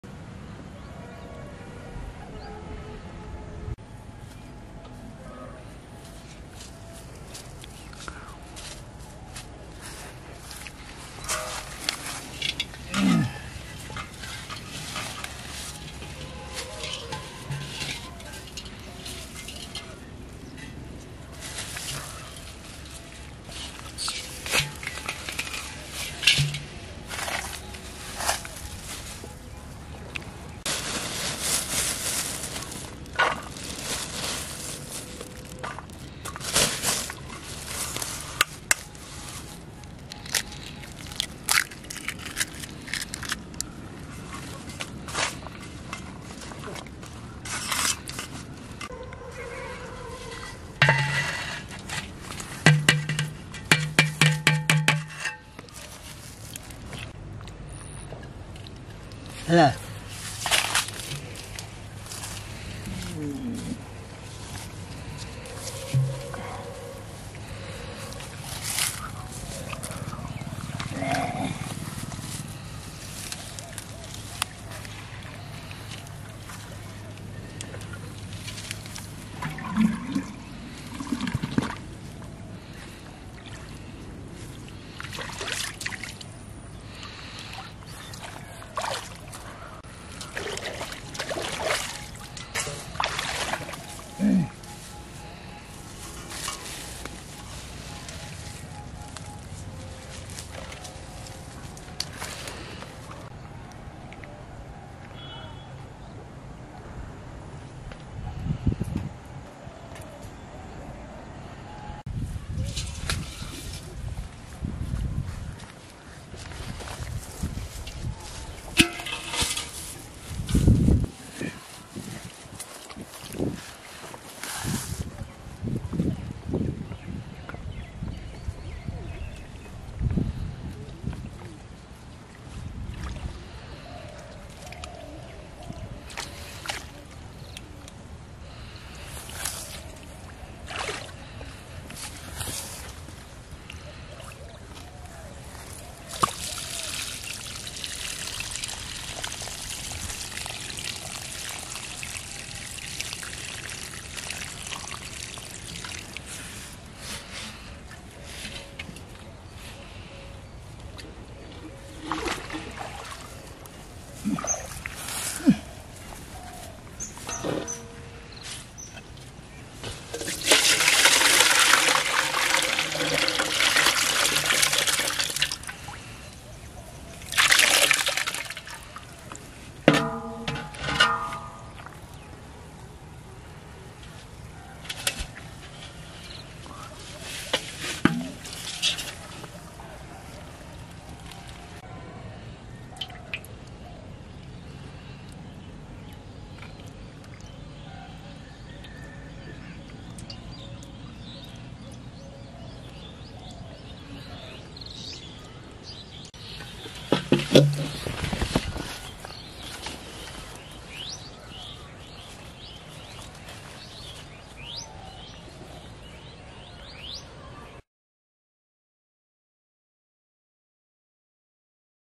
New Method | Grandpa taught me how to catch fish with a clay pot | মাটির হাড়ি দিয়ে মাছ ধরার শিখালো
Ajay River in a new trick